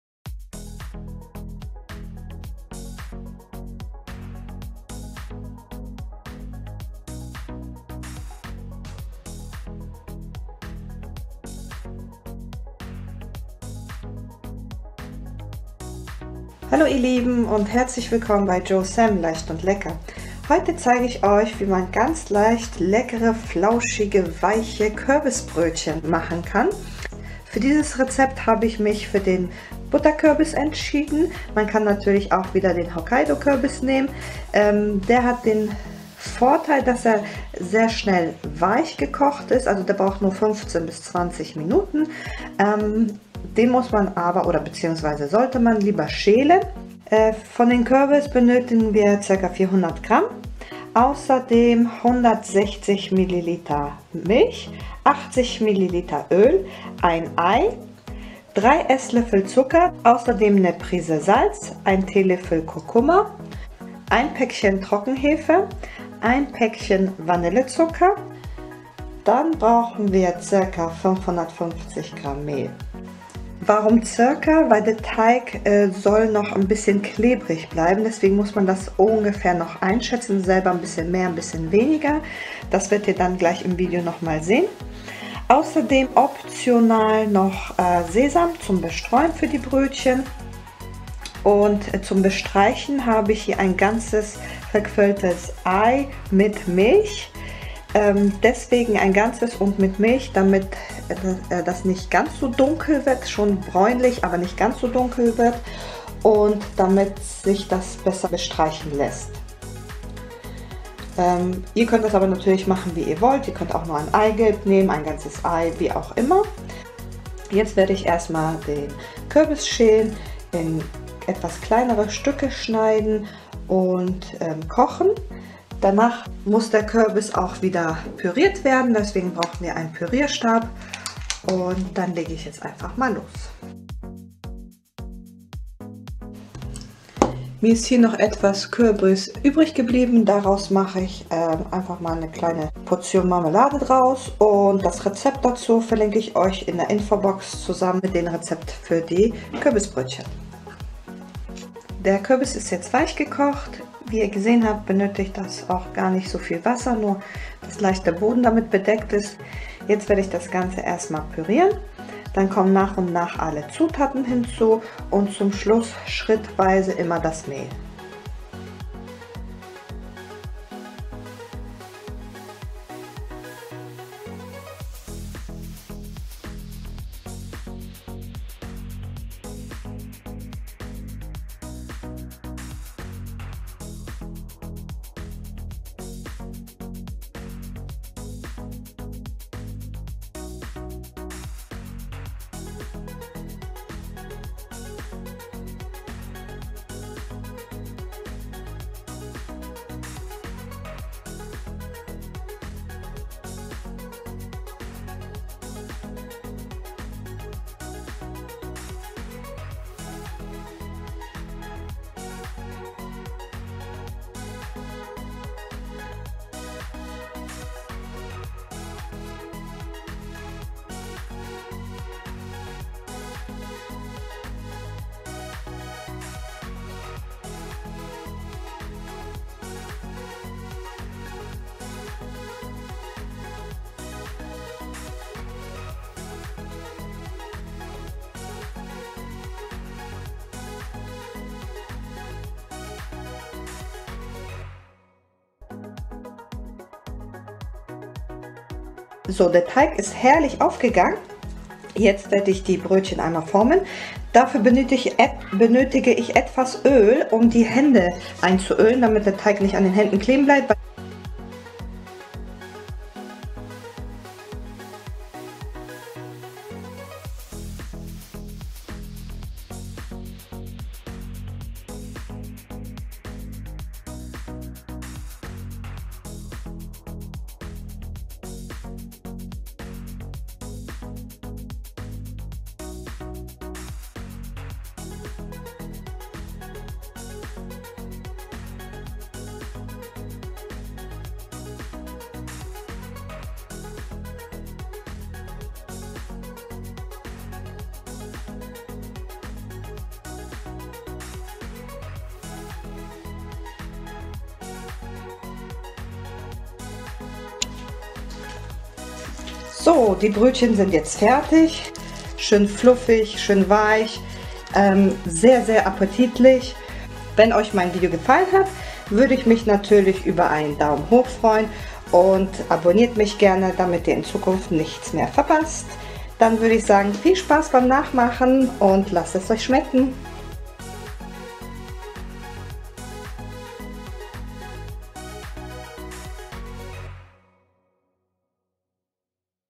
Super weiche Kürbisbrötchen 🎃 ohne zu kneten, für die ganze Familie, leicht & lecker
Wer kennt nicht die flauschig-weiche Kürbisbrötchen vom Lieblings Becker. Die sehen immer so gut aus, dass man sich immer ein mitnimmt. Aber nirgends schmeckt es so gut wie zu hause. Deswegen ran an die Schüssel und selber machen!
Passend zur Jahreszeit, gibt es heute ein leckeres Rezept für fluffige Kürbisbrötchen. Mit 🎃 Kürbis kann man ja so einiges machen, warum nicht auch flauschige Brötchen. Die Farbe steht den Brötchen wirklich gut und lässt sie super appetitlich aussehen.
Ansonsten sind die Brötchen schön weich und fluffig. Erinnert von der Konsistenz ein wenig an Milchbrötchen, sind aber nicht süß. Wer süße weiche Kürbisbrötchen möchte, sollte unbedingt mehr Zucker oder andere Süßungsmittel an den Teig geben.

Zutaten für ca. 9 Brötchen
- ca. 300-400g Butterkürbis
- 160ml Milch
- 80ml öl
- 1 Ei
- 3 EL Zucker
- 1 Prise Salz
- 1 TL Kurkuma
- 1 Päckchen Vanille Zucker
- 1 Päckchen Trockenhefe
- ca. 550-650g Weizenmehl 405er (eventuell noch etwas mehr)

Außerdem
- etwas Sesam
- 1 Ei mit 1 EL Milch zum bestreichen

* Bei 180° Ober- und Unterhitze ca. 20 Minuten Goldbraun backen *

Kürbis Marmelade
Zutaten :
- 380g Kürbisfleisch
- 1 Apfel
- 1 Orange, ausgepresst
- 1 prise Zimt
- 1 Sternanis
- 250g Gelierzucker 2:1

Orange aauspressen, Kürbis und Apfel schälen und in kleine Stücke schneiden. Den Kürbis und Apfel mit dem Orangensaft auf mittlerer Stufe ca. 15 Minuten weichkochen. Anschließend mit einem Mixstab pürieren, dann Gelierzucker, Zimt und Sternanis hinzufügen und für weitere 5 Minuten sprudelnd kochen, dabei immer wieder umrühren.
Dann die Masse noch heiß in saubere abgekochte Gläser füllen und sofort verschließen.